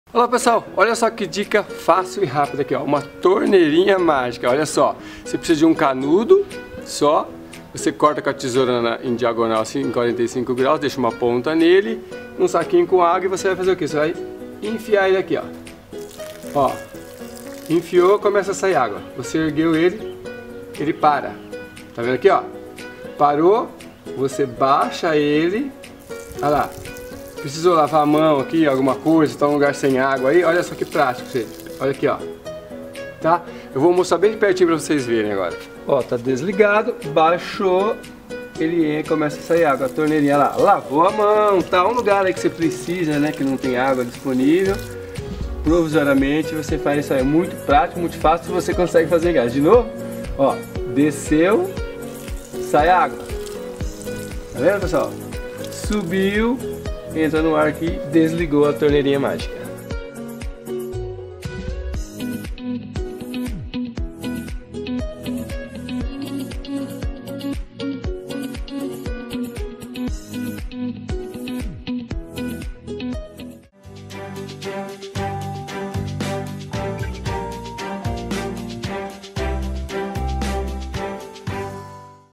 Torneirinha Mágica
Olha como é fácil fazer
Em alguns momentos pode ser útilem lugares que não tem água.
Obrigado